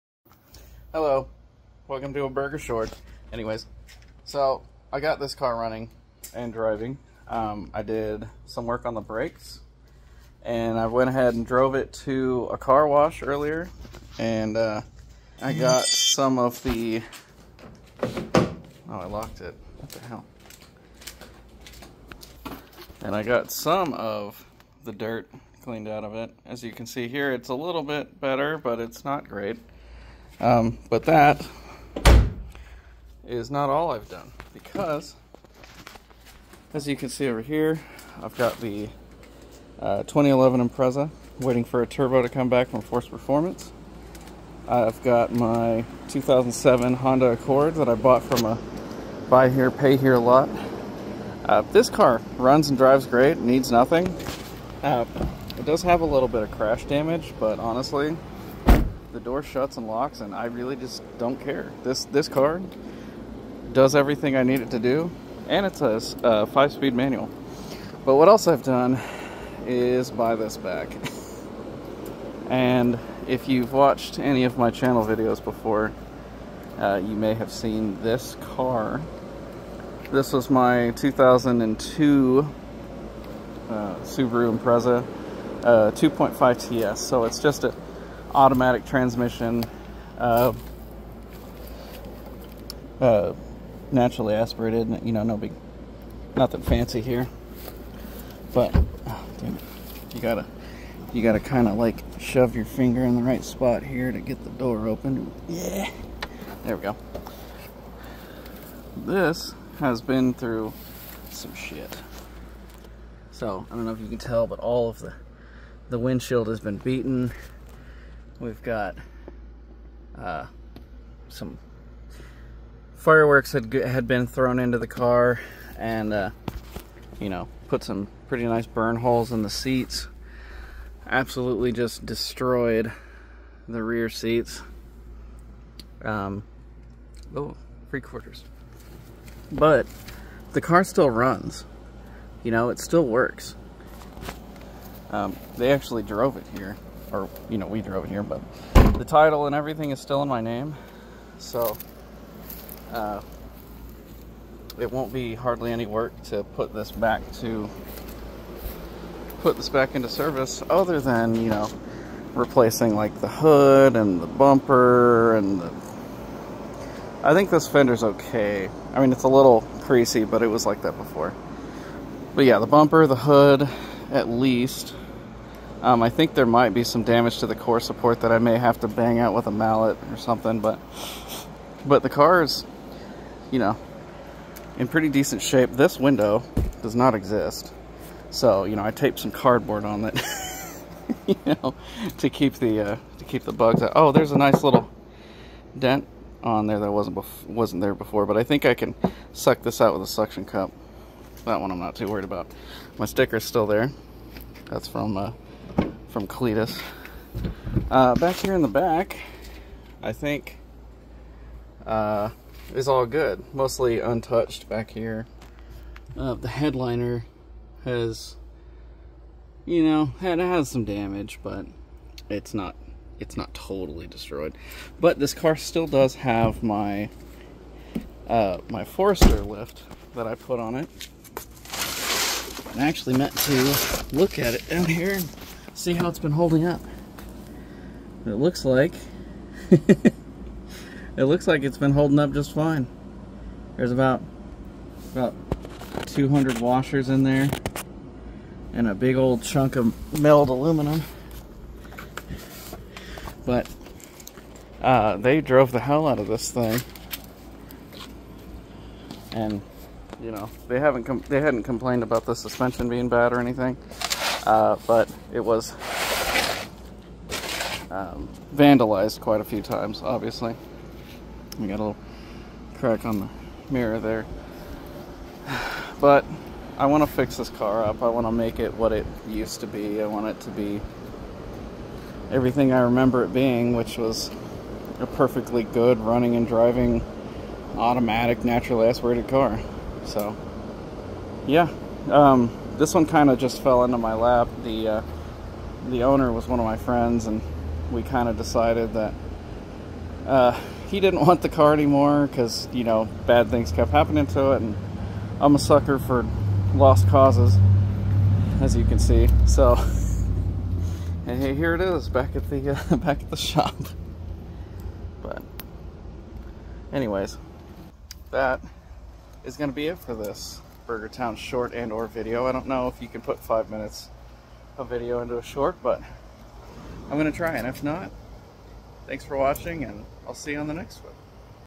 Oops I did it again, again. 2002 Subaru Impreza 2.5TS restoration, battle damaged
This car has had its fair share of abuse and I simply couldn't allow it to be scrapped or destroyed further. being my first engine rebuild, I felt it deserved way better than it was getting. I'm ready to spend way too much money making this right again.